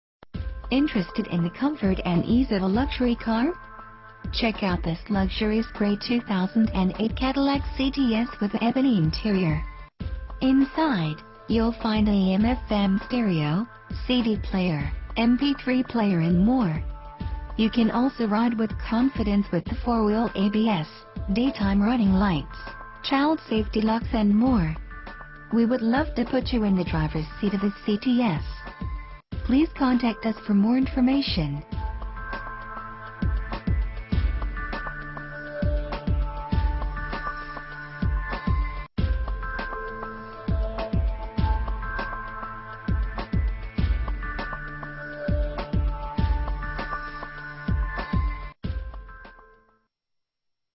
Used 2008 Cadillac CTS Colorado Springs CO - by ...
- Used 2008 Cadillac CTS VIN 1G6DF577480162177 from Phil Long Ford Motor City by EveryCarListed.com Interested in the comfort and ease of a luxury car? Check out this luxurious Gray 2008 Cadillac CTS with a Ebony interior. Inside, you'll find a AM/FM Stereo, CD Player, MP3 Player and more. You can also ride with confidence with the 4-Wheel ABS, Daytime Running Lights, Child Safety Locks and more. We would love to put you in the driver's seat of this CTS.